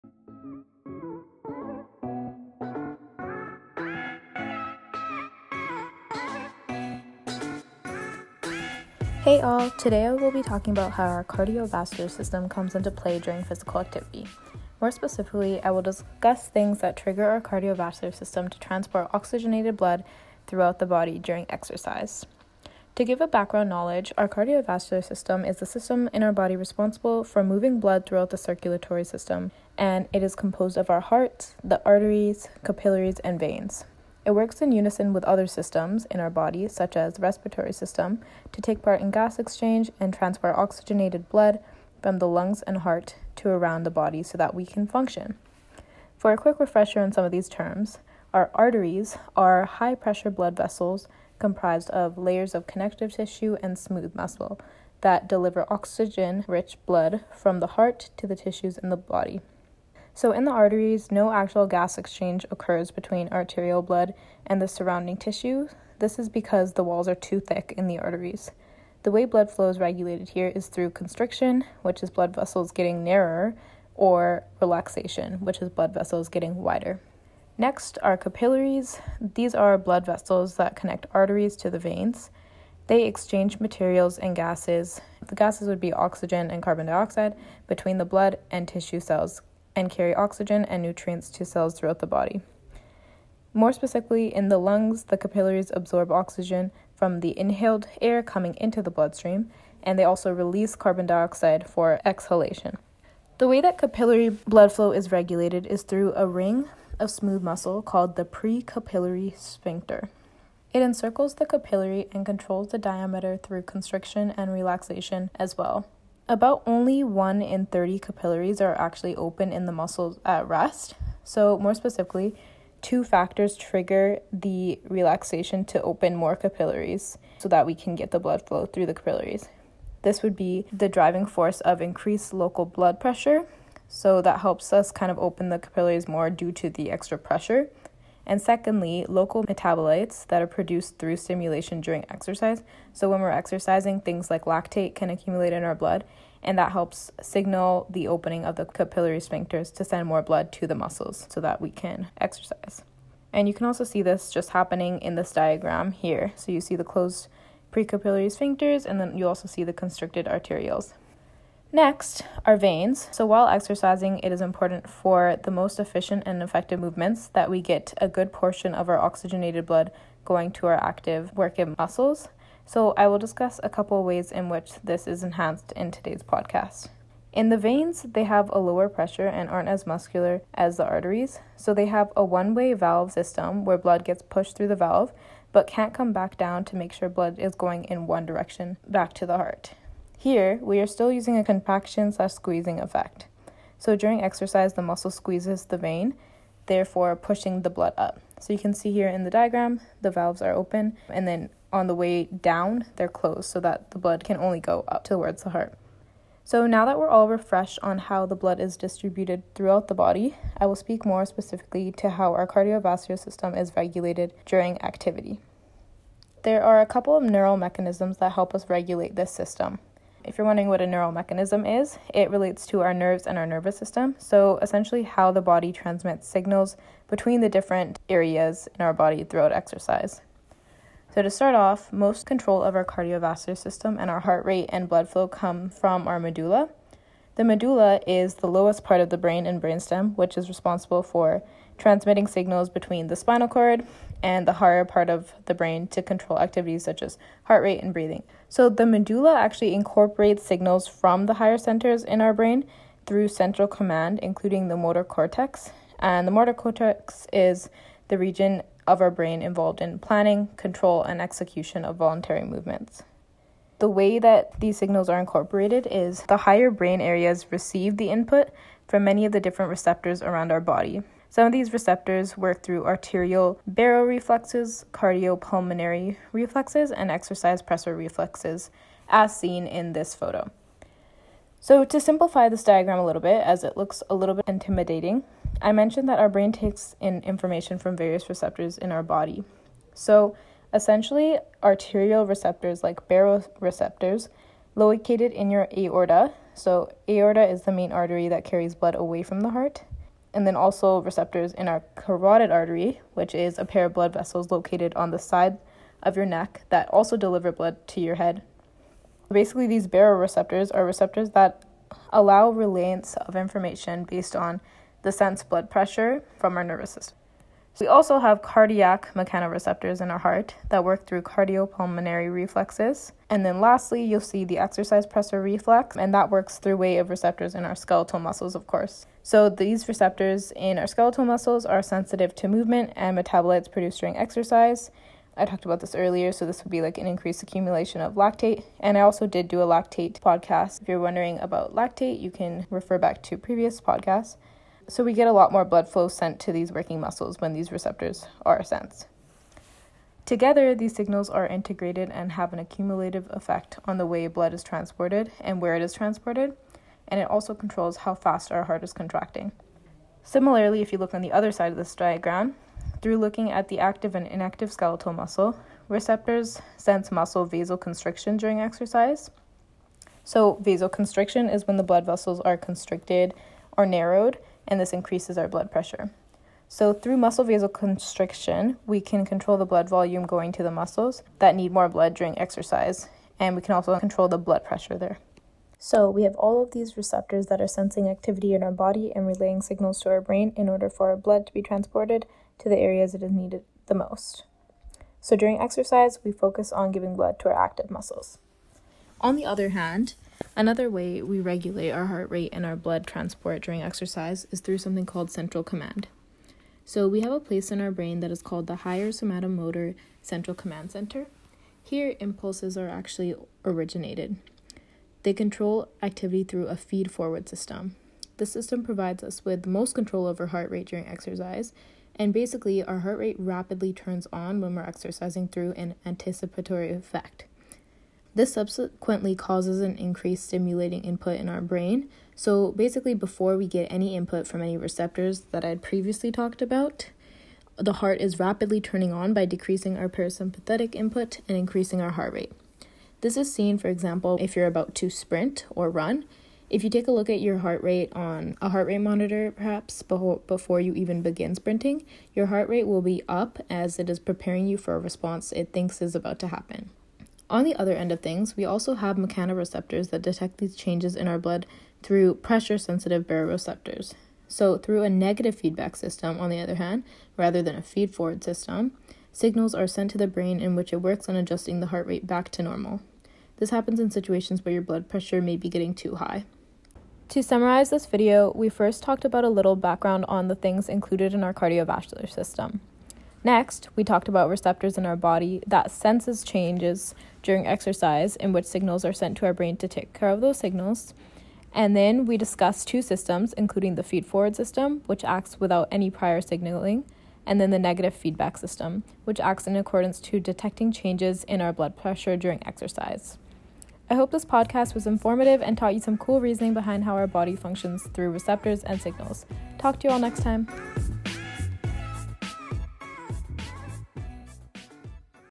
How Exercise Affects the Cardiovascular System - Exercise Physiology Podcast Ep. #4
Have you ever noticed that when you’re about to sprint, your heart rate increases before you’ve even begun? This is all because of the Higher Somatomotor Central Command Centre in our brain! 🧠

So how do our bodies sense these changes and what changes occur in our body?

Topics covered in this Episode, click on the timestamps below to jump to that topic!
(0:09) - Introduction
(0:23) - Overview of Respiratory System
(03:32) - How our Cardiovascular System is Regulated during Activity
(06:45) - Central Command

 🏢 201-2520 Ontario St., Vancouver
 🏢 101-1125 Nicola Ave., Port Coquitlam